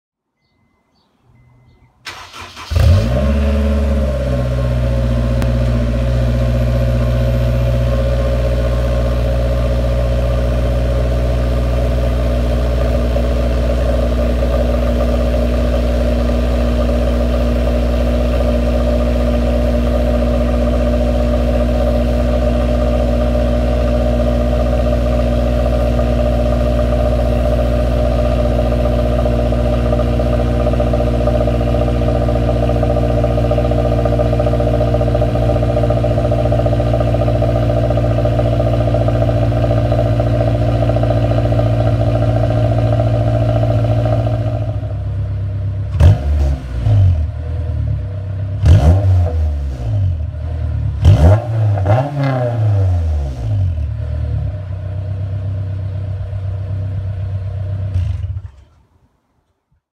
S2000 Greddy SE Catback and RSG HFC
Ap1 with a greedy spectrum elite catback and a rockstar garage high flow cat